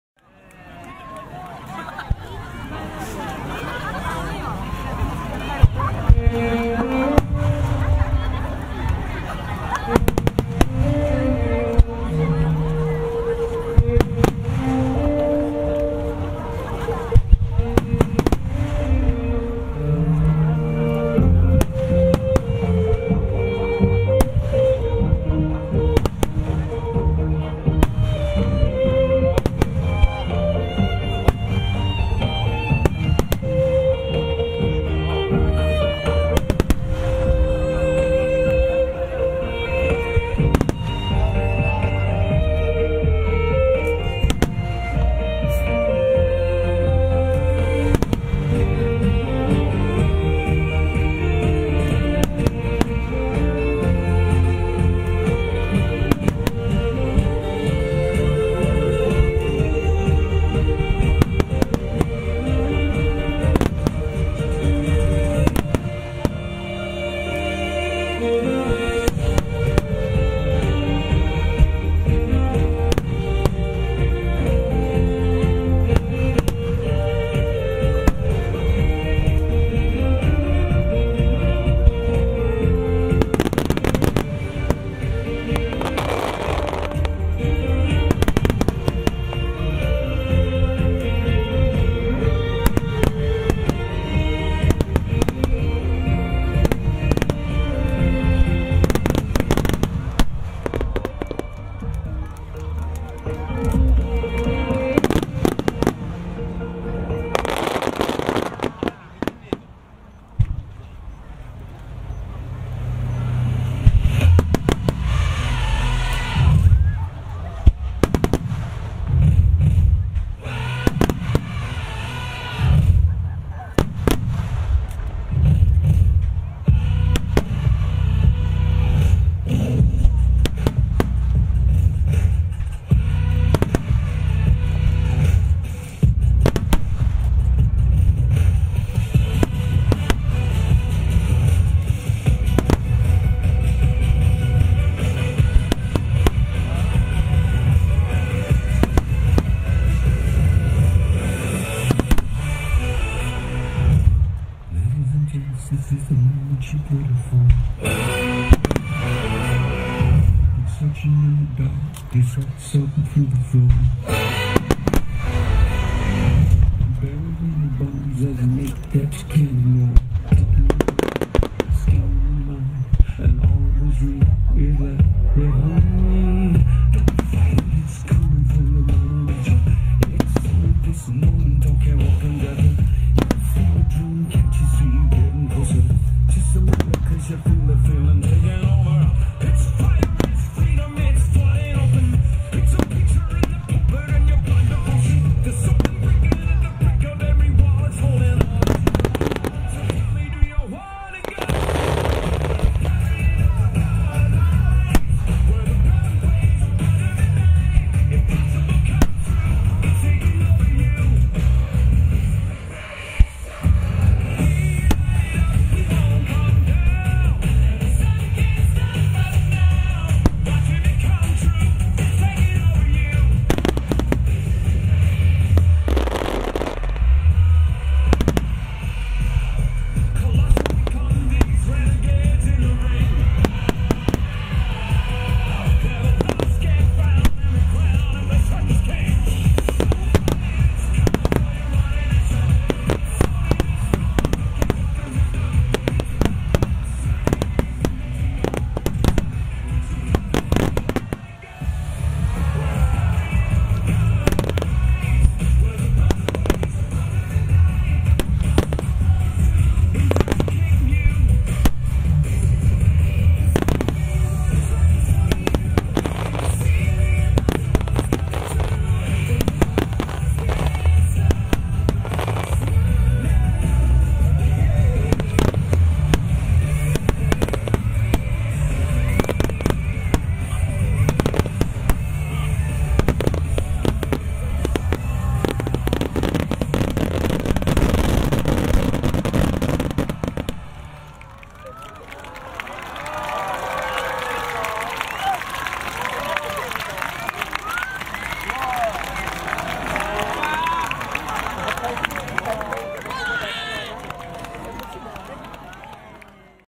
ぷちでらっくすさん グランドフィナーレ It's Show Time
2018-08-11 茨城県常総市　「第54回常総きぬがわ花火大会」野村花火工業のミュージックスターマイン